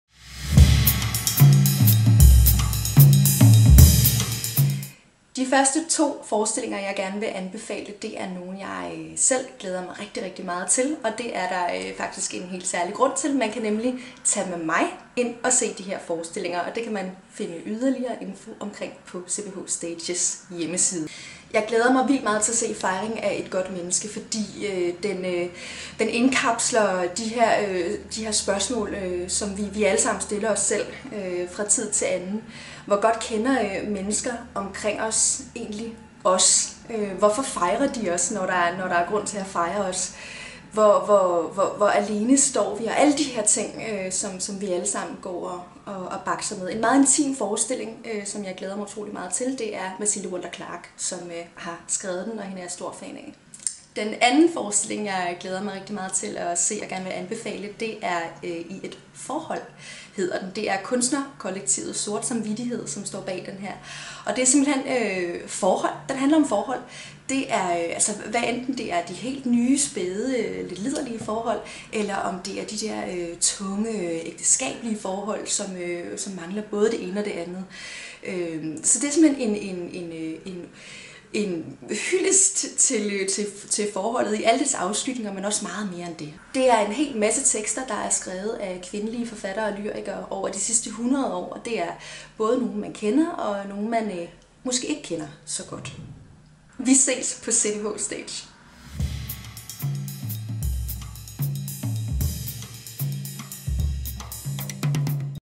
Katherine Diez anbefaler
Litteraturanmelder Katherine Diez anbefaler 1 event med 2 forestillinger under CPH STAGE 2019: